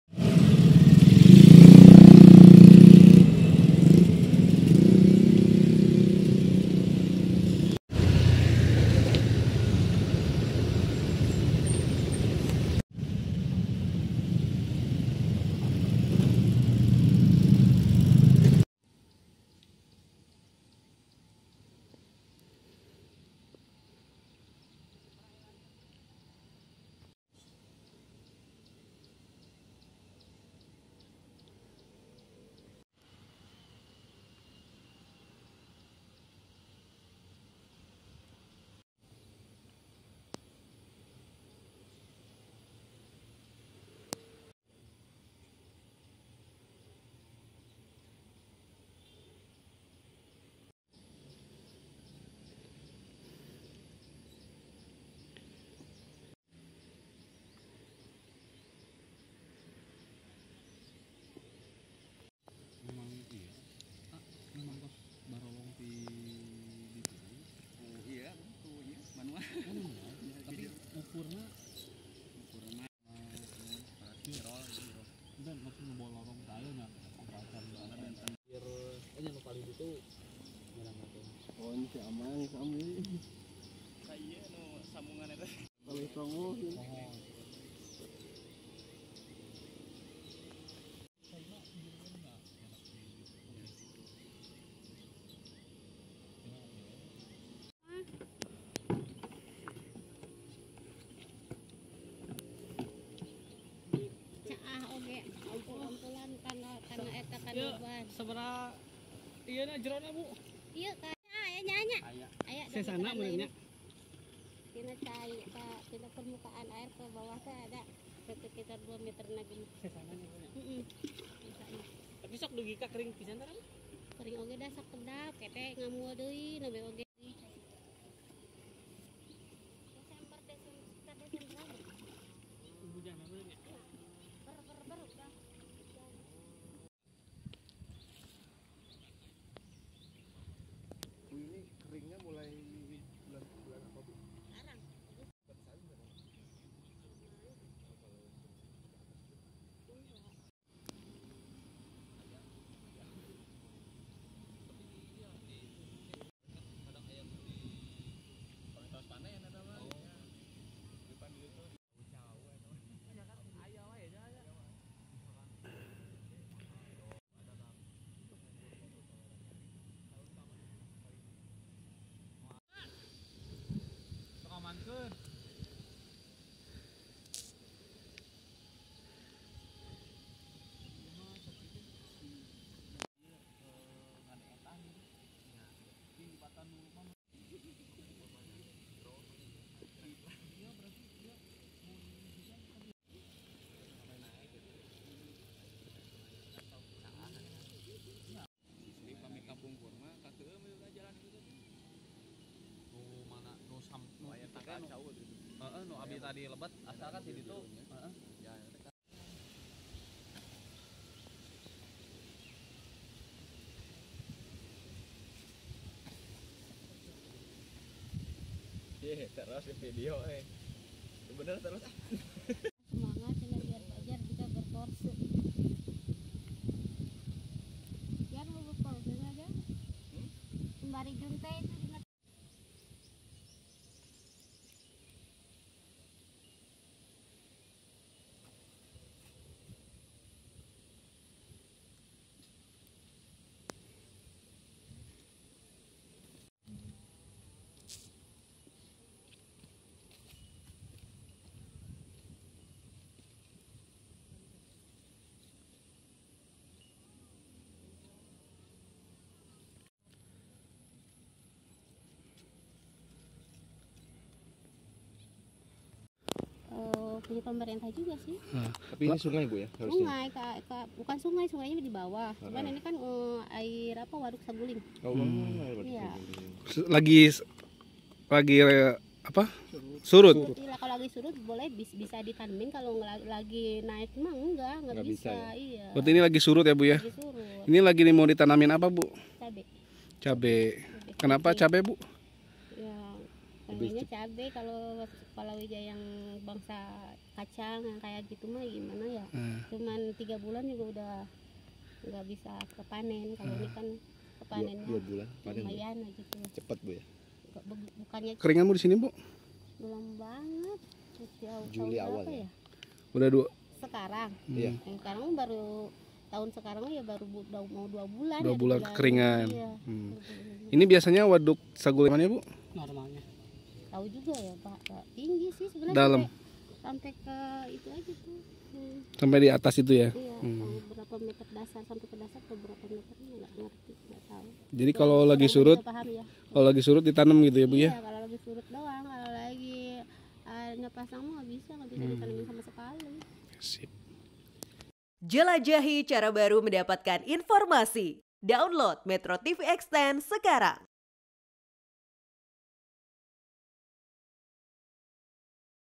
Manfaatkan Kekeringan Waduk Saguling Warga Tanam Cabai
MetroTV, Musim kemarau yang berkepanjangan menyebabkan aliran Waduk Saguling di Desa Mukapayung, Cililin, Kabupaten Bandung Barat, mengering. Hal ini dimanfaatkan sejumlah warga untuk menanam tanaman yang layak ditanam/hasilnya untuk kebutuhan sehari-hari.